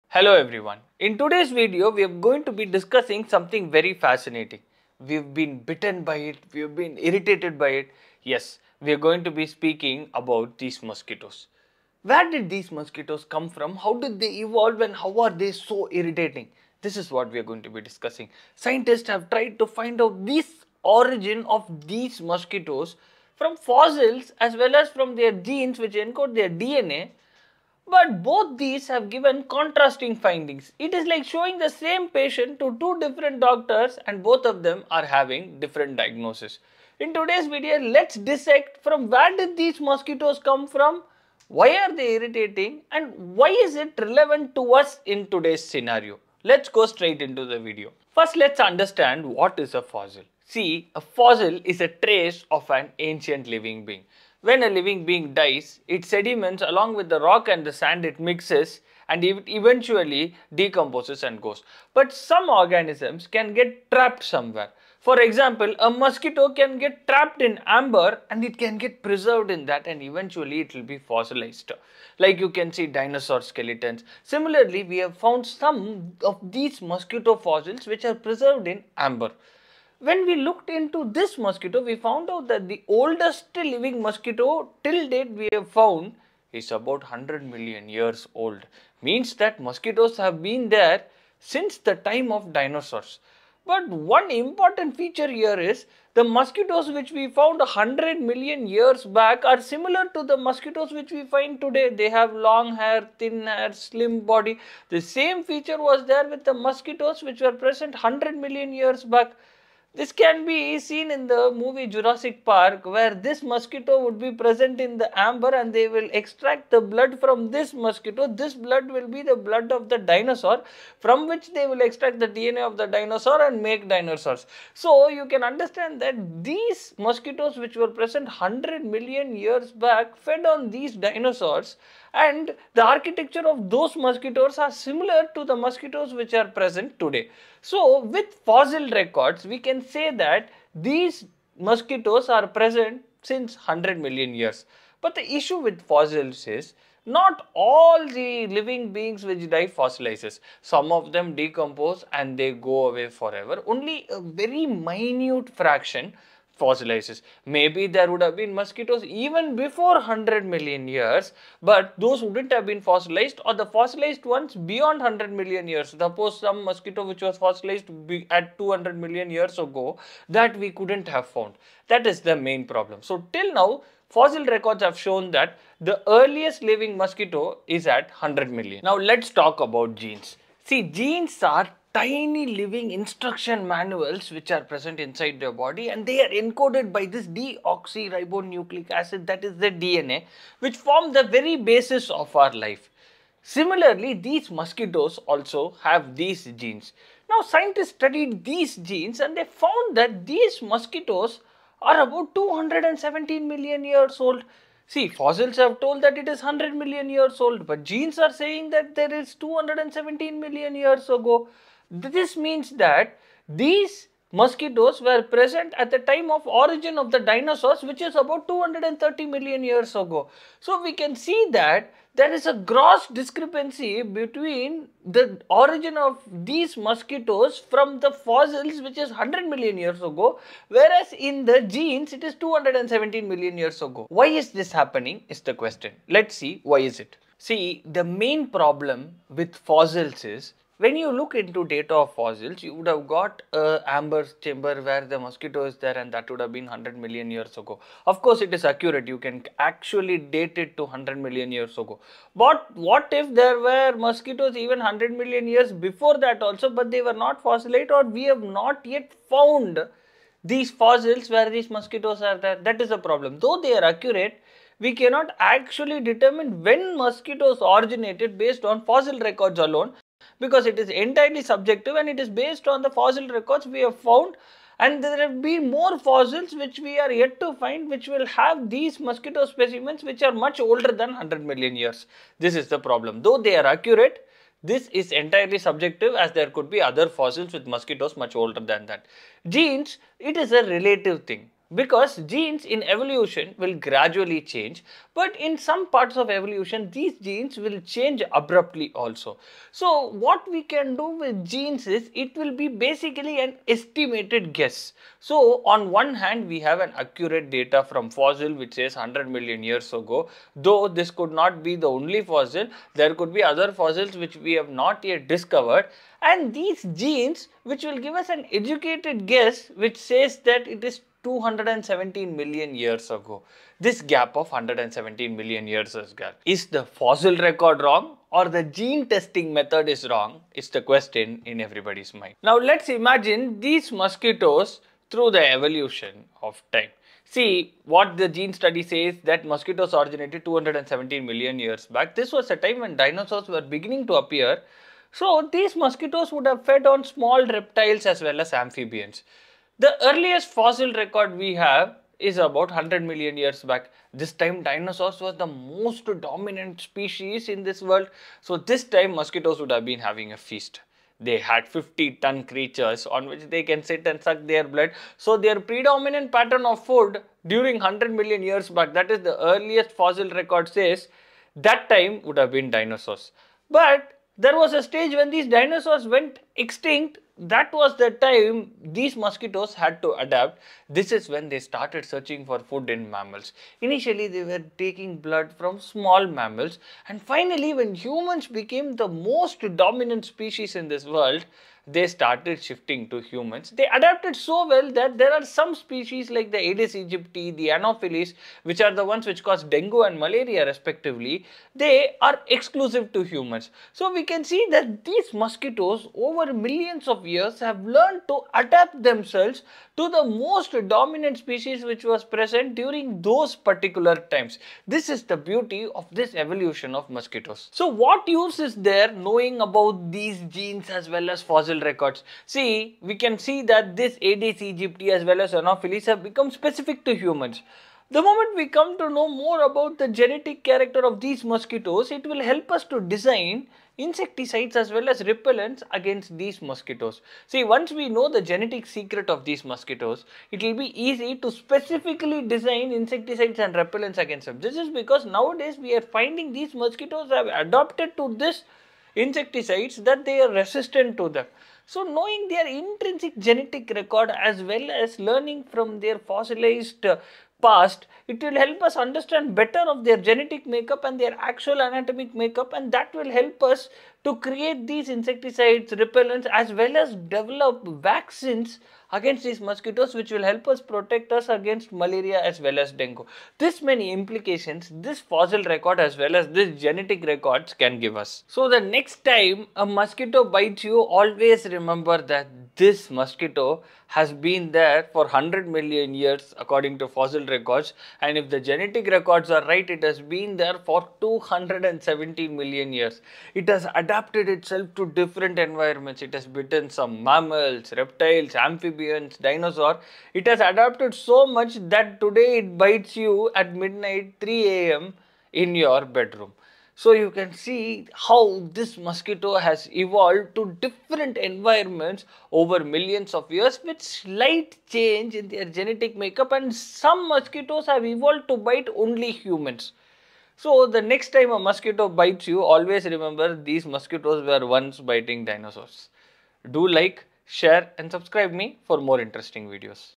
Where Did Mosquitoes Come From? The Fossil vs. Genome Debate
Join us for a fun and fascinating journey into the world of mosquito origins! Discover how scientists are using fossils and DNA to unravel the mystery of when mosquitoes first appeared on Earth. We’ll explore why fossils and genes give different answers, what this means for mosquito evolution, and how understanding their history helps us fight diseases like malaria and dengue. Packed with humor and easy-to-understand science, this talk is perfect for anyone curious about the tiny insects that have been around for over 200 million years—and how they became the masters of survival and annoyance.